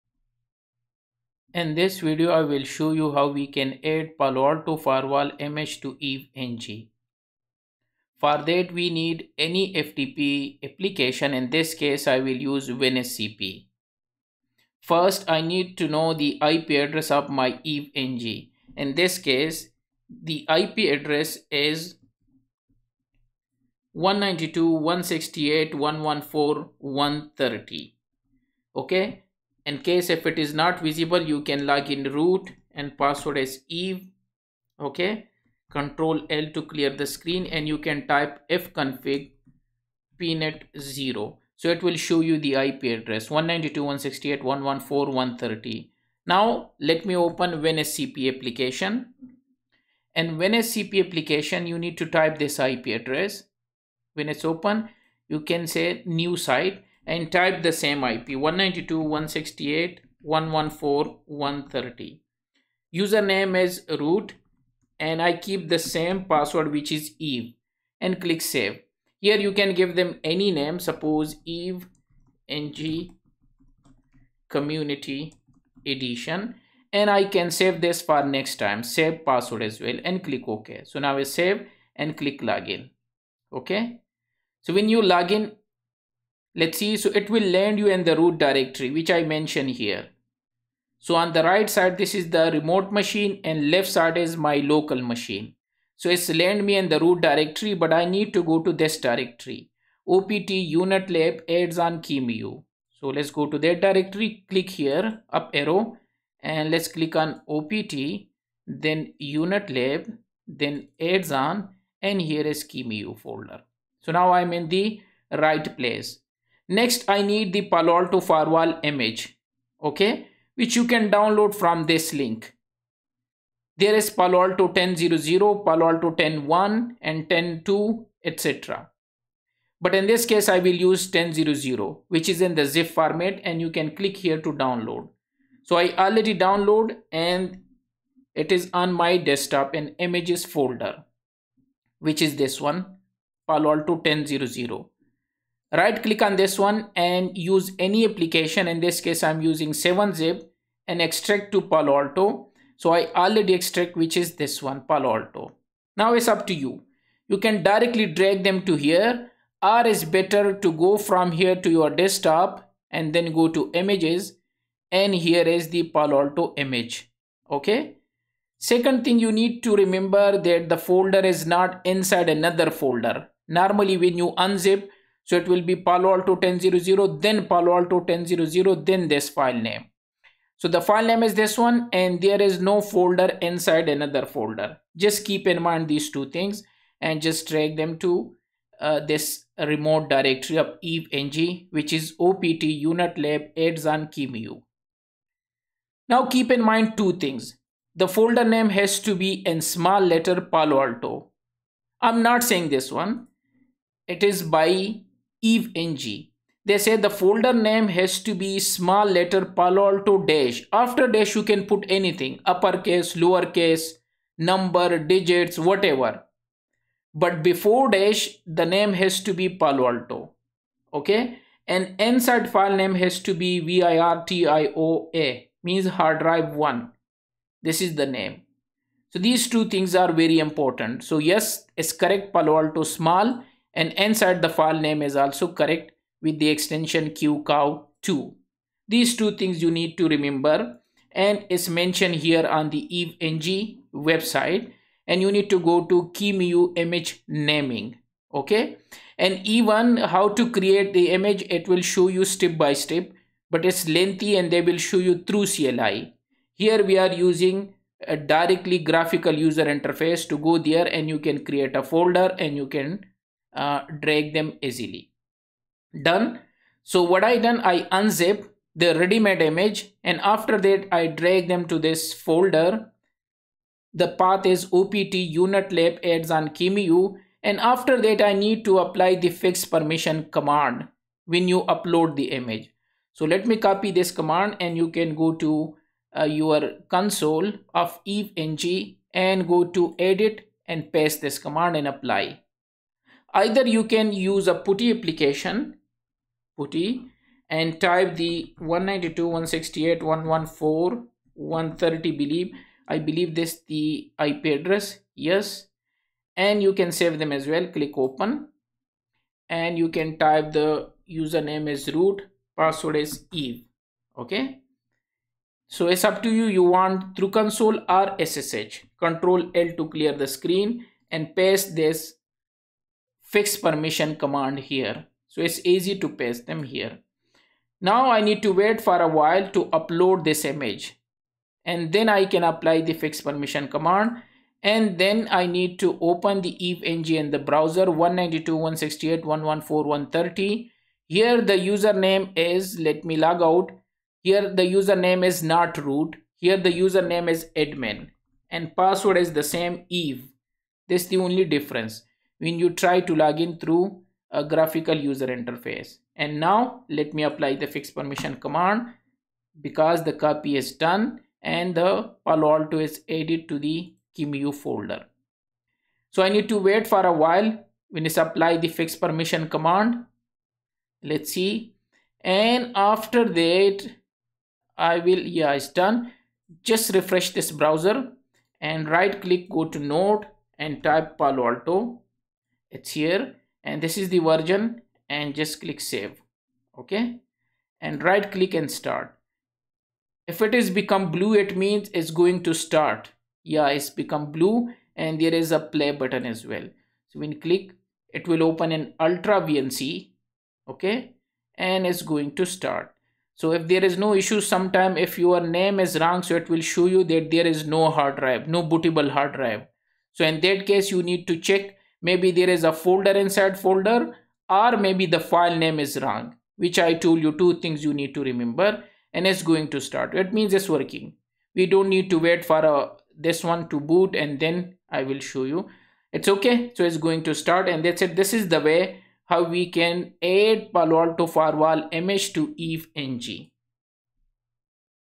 Add PaloAlto Firewall 10 Image in EVE NG | PCNSE
In this video, we will see how to import Palo Alto Firewall image into EVE-NG Simulation Software and get them to work.

Watch this video till the end and drop your views in the comment section below.